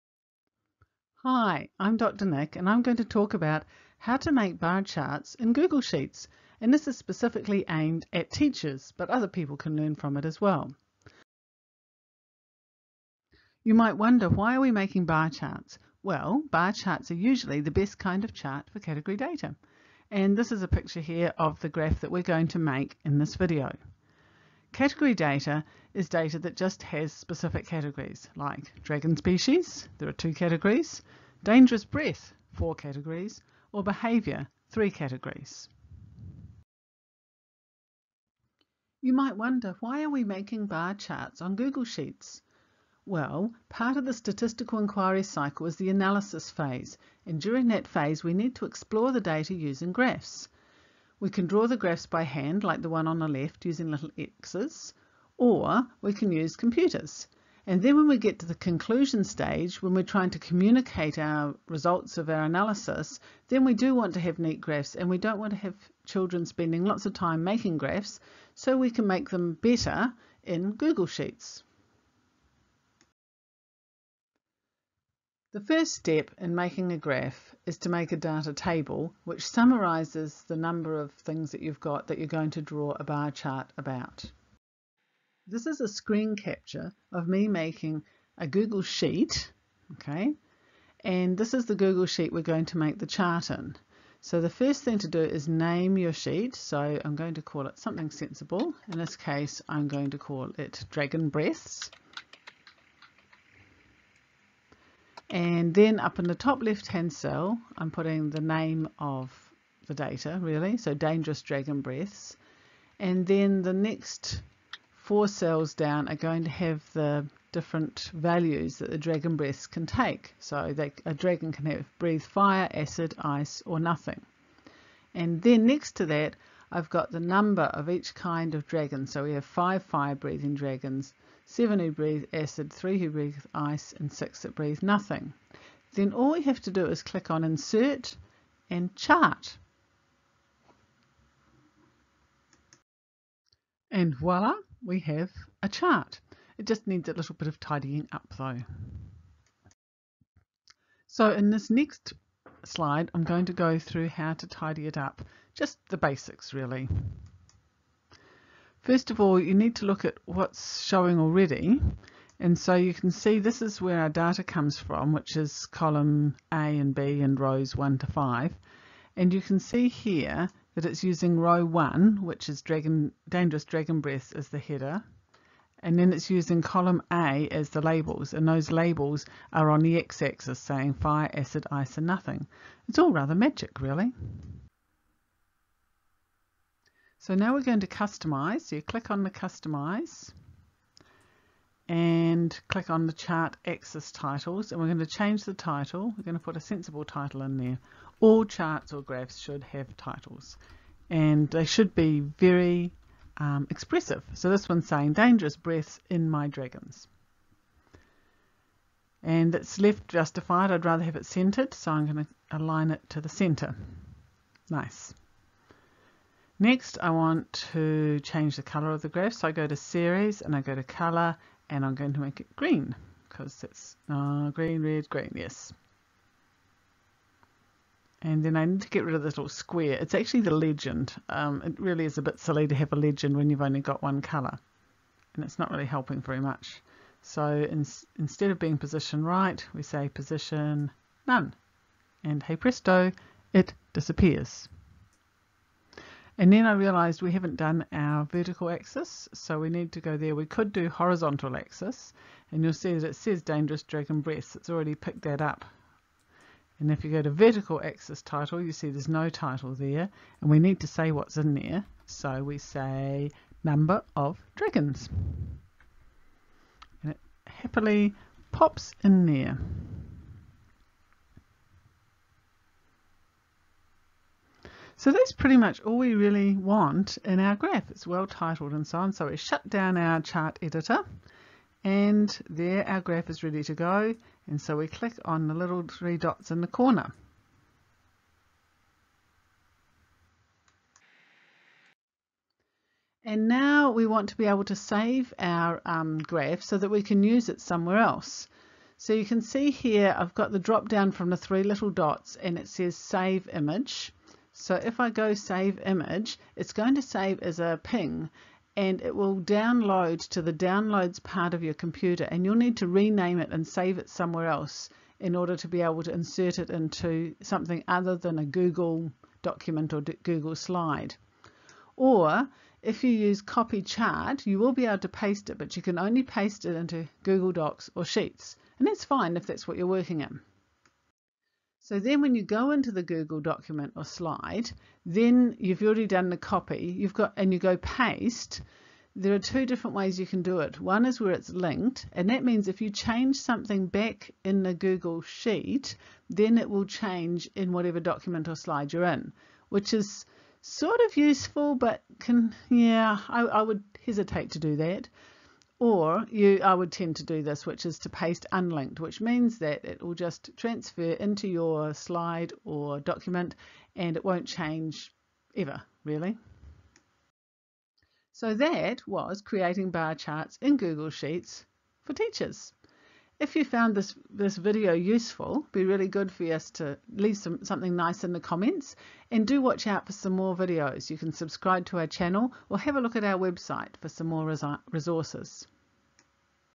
Spreadsheets with Dr Nic: Making Bar Charts in Google Sheets
It is amazingly straight-forward to make bar charts using Google sheets. The trickiest part is getting them into other documents. Dr Nic shows how to make a bar chart of Dragonistics dragon breaths.